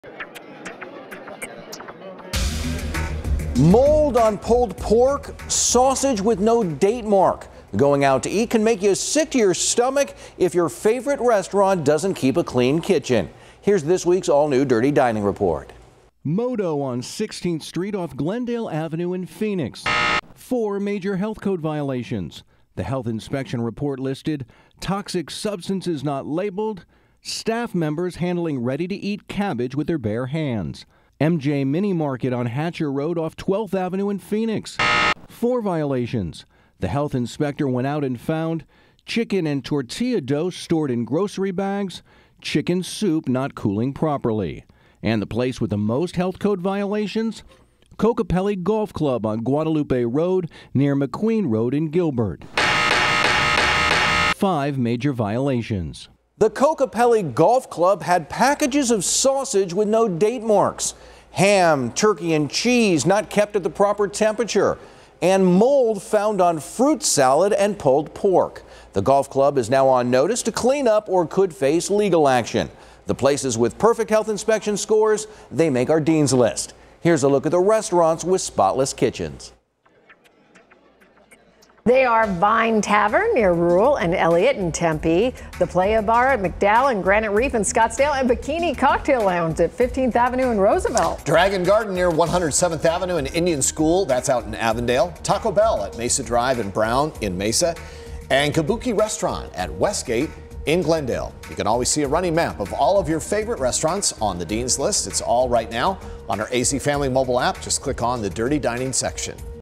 Health inspectors ding several restaurants in this week's Dirty Dining
Going out to eat can make you sick to your stomach if the kitchen isn't cleaning. What health inspectors found in this week's Dirty Dining.